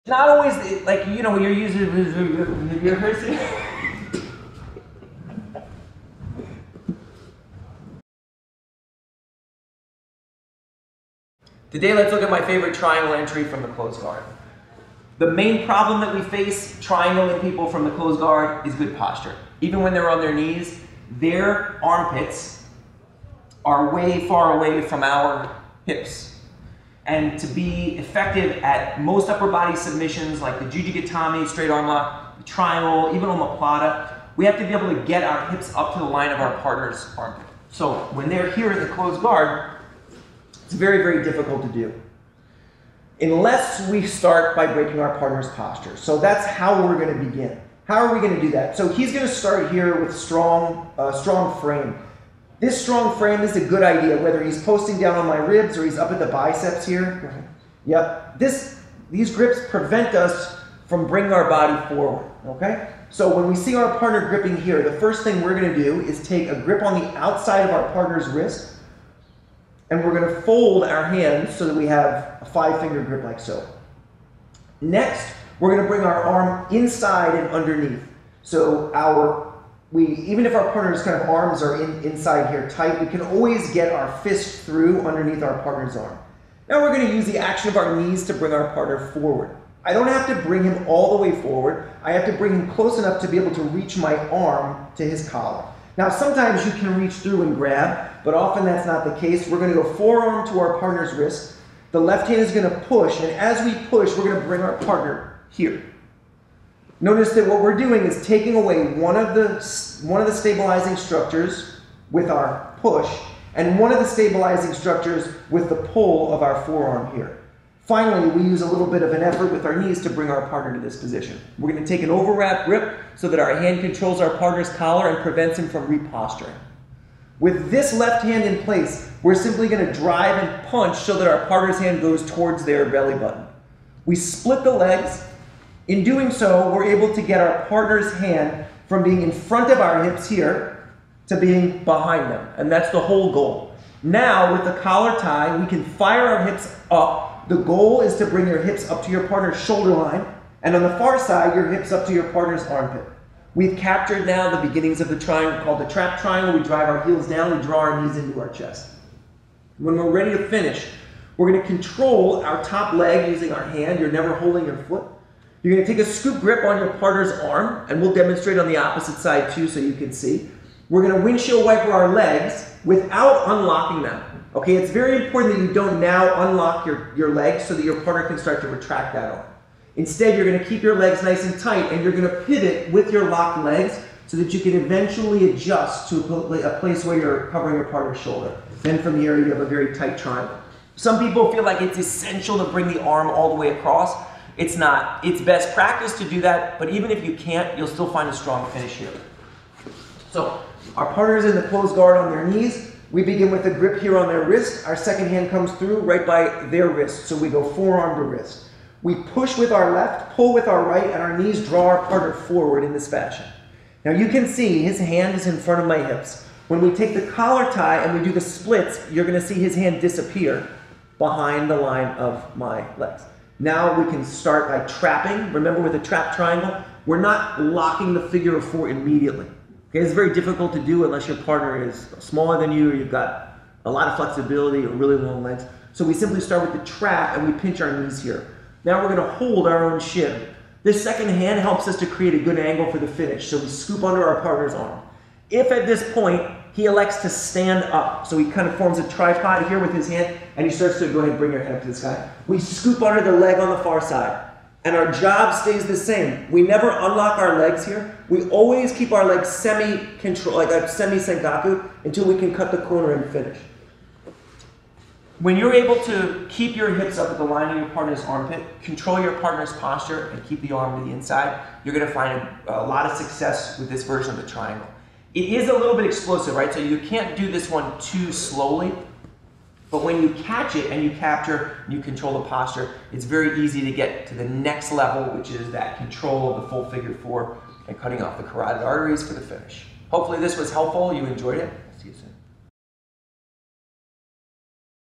No Gi Triangle from Closed Guard: My Favorite Variation
My favorite no gi triangle: without the use of the gi collar and sleeves, no gi closed guard entries face a few difficulties but none of these should prevent you from making the triangle a staple attack from your closed guard. Our entry is a relatively simple, “tried-and-true” method with a focus on a few key details. We use a set of core principles seen across most triangle variations: we begin with wrist control and deal with the ever-present problem of posture by taking an early grip at the wrist and removing the posting arm. Then we use kazushi to disguise our attack, relying on movement and some speed to close the trap triangle. From there we advance to full control with the complete triangle.